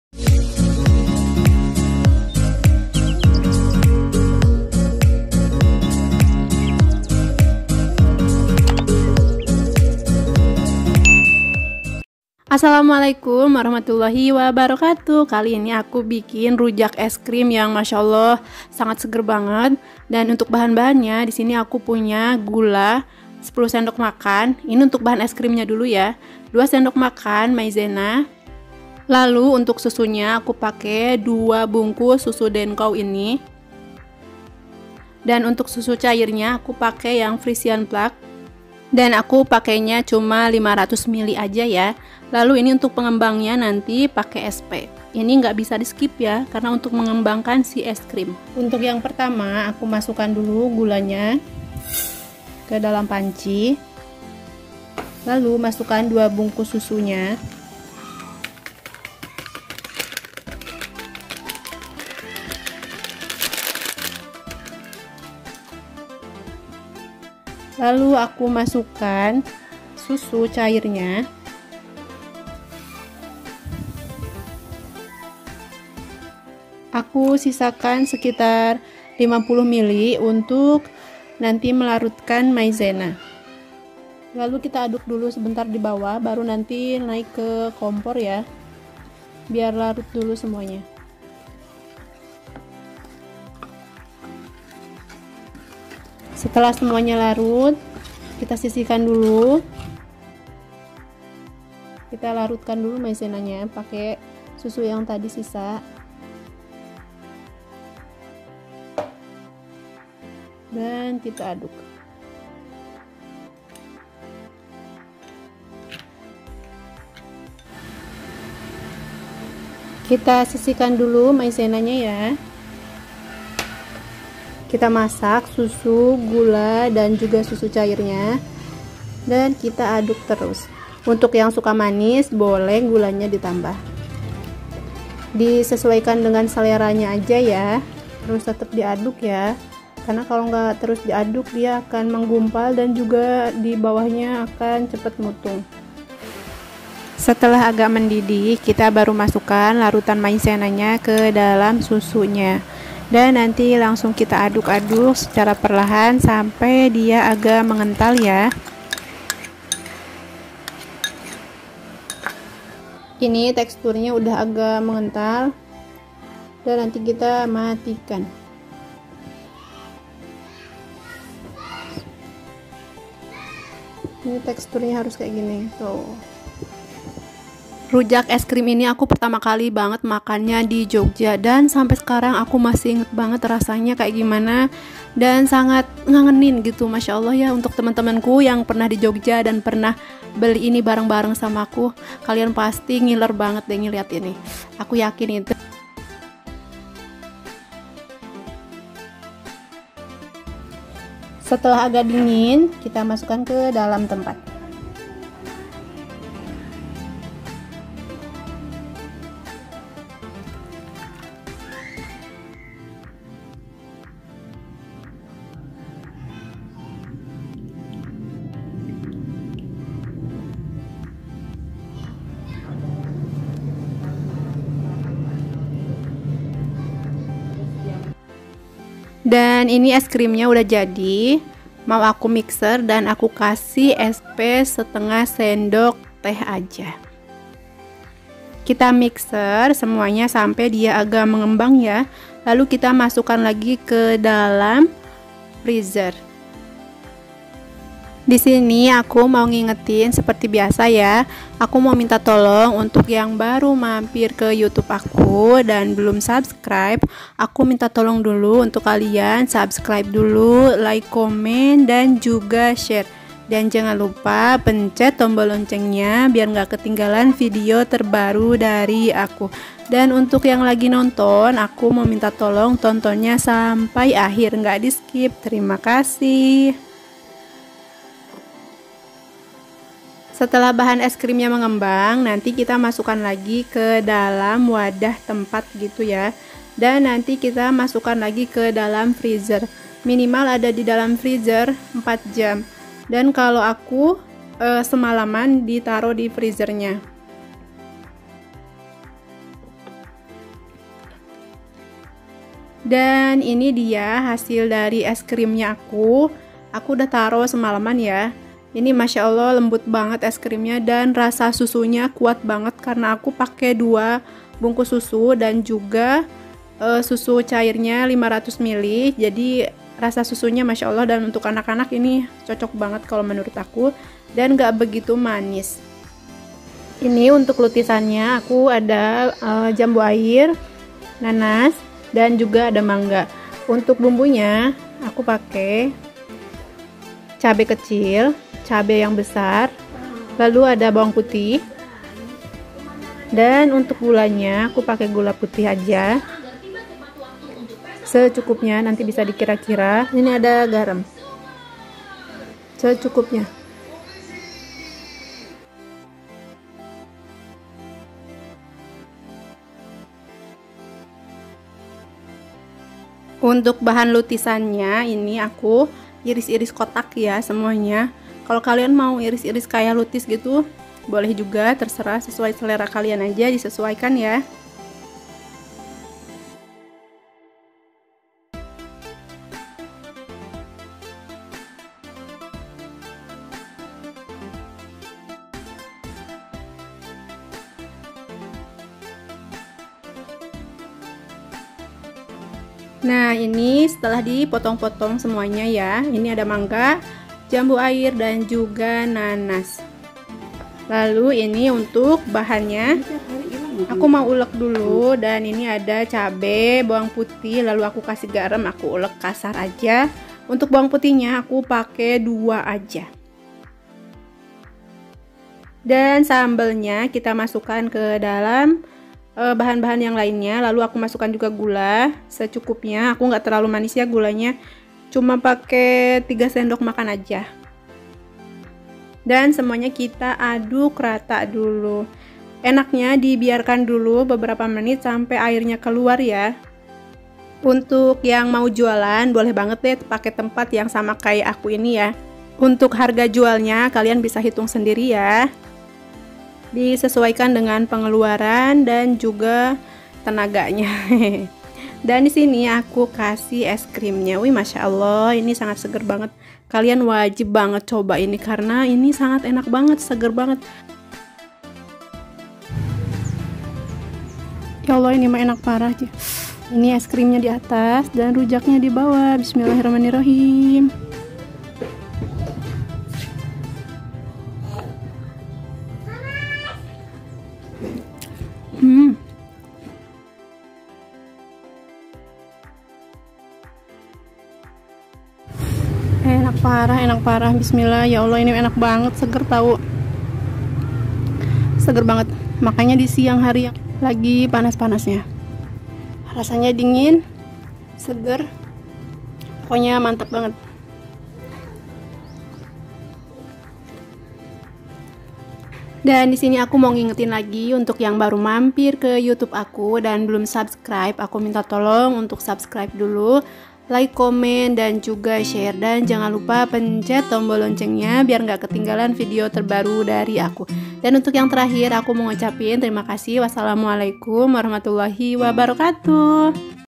RUJAK ICE CREAM
RESEP ice cream

susu bubuk 2 bungkus
susu uht 500 ml
sp 1/2 sdt
maizena 2 sdm

bahan lutisan
nanas
mangga muda
jambu air

cabe rawit
cabe kriting

untuk cara buat silahkan tonton  video nya ya

camera video pake hp samsung a30s
edit video kinemaster
musik dari youtube
tripod pake merk xiomi